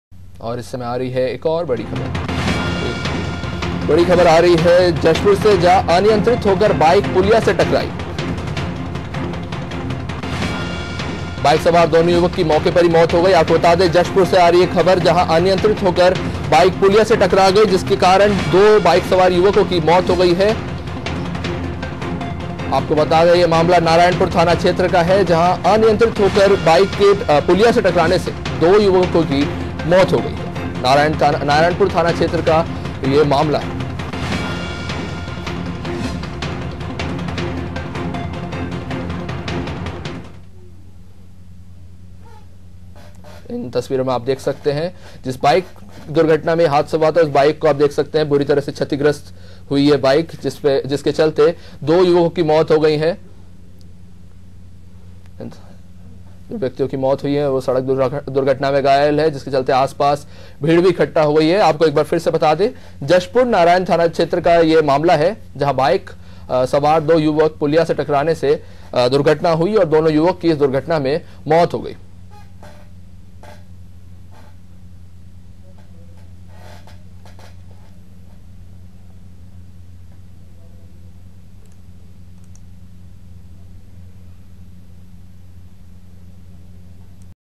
BREAKING |जशपुर : अनियंत्रित होकर बाइक पुलिया से टकराई | ANAADI TV
जशपुर अनियंत्रित होकर बाइक पुलिया से टकराई बाइक सवार दोनों युवकों की मौके पर मौत अनियंत्रित होकर बाइक पुलिया से टकराई नारायणपुर थाना क्षेत्र का मामला